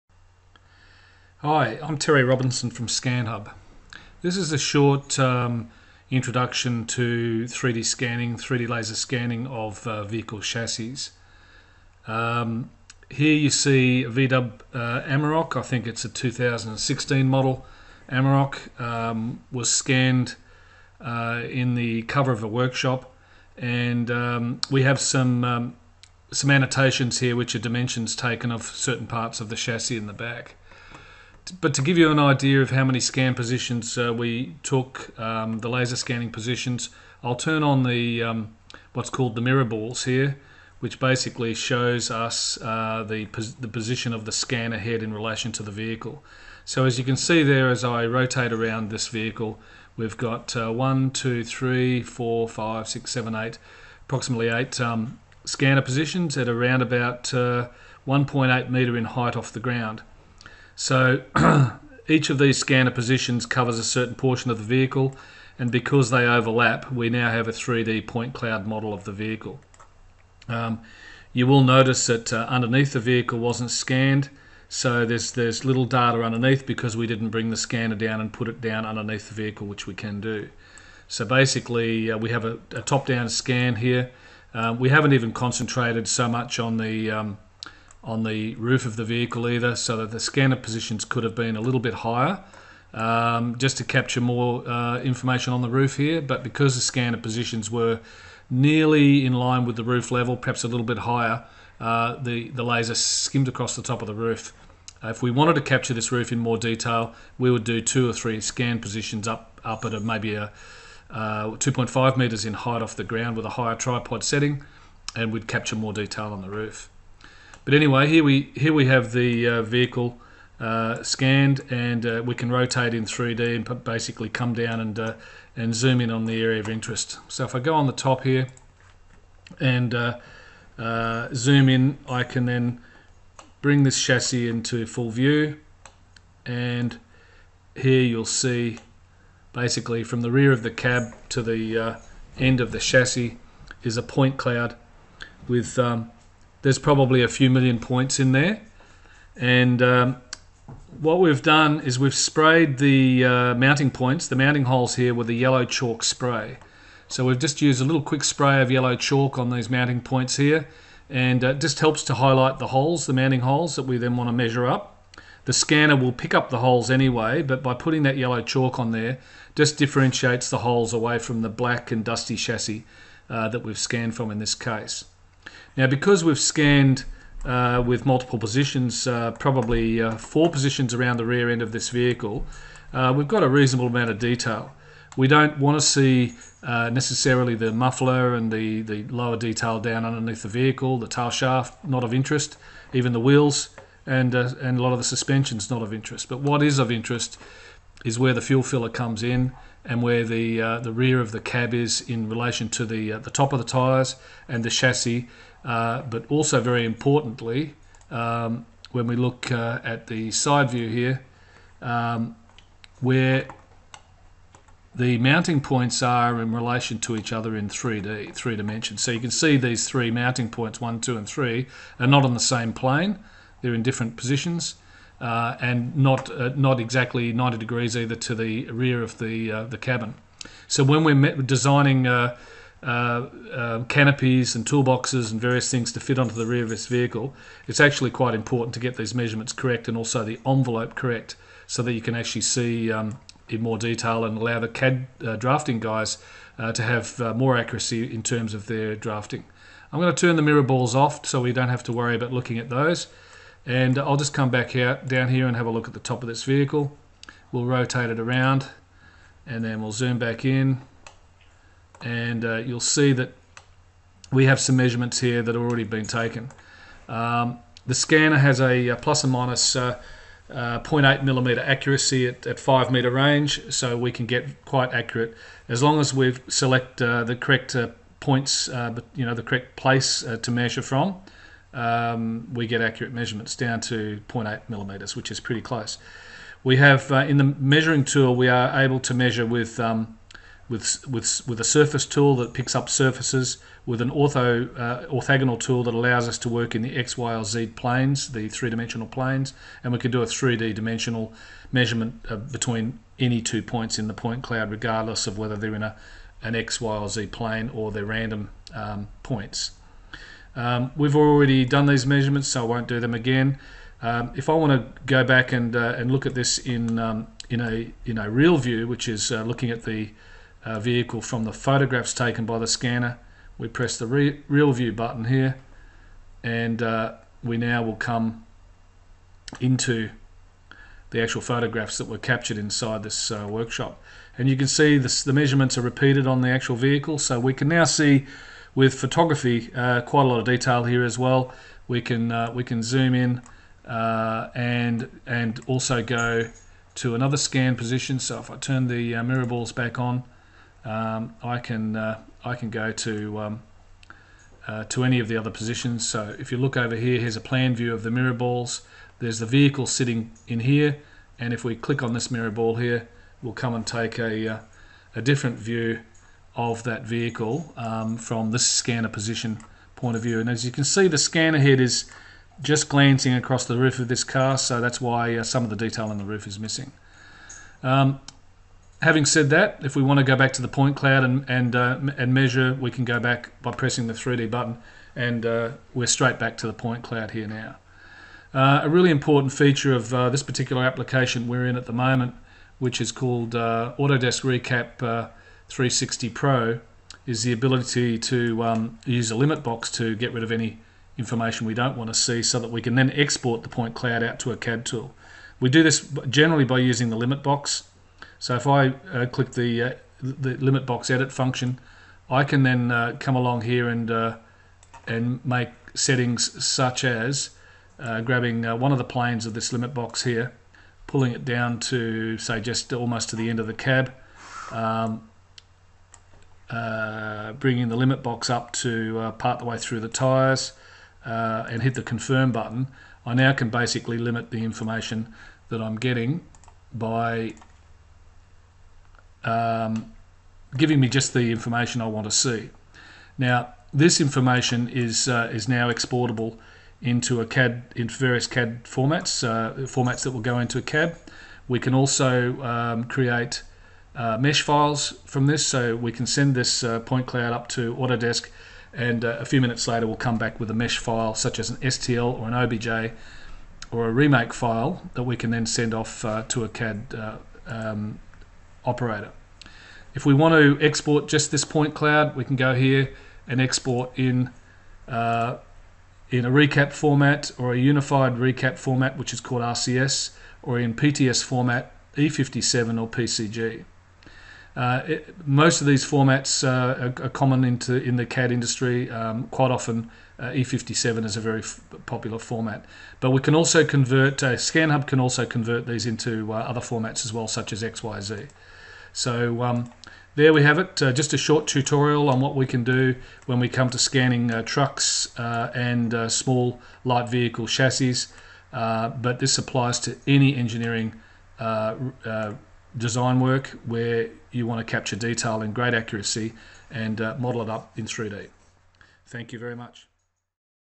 3D Laser Scanning of light vehicle chassis
For SUV and truck tray and canopy body builders and designers. 3D Laser Scanning is accurate and saves time and cost in the engineering process from measurement through concept and  design documentation.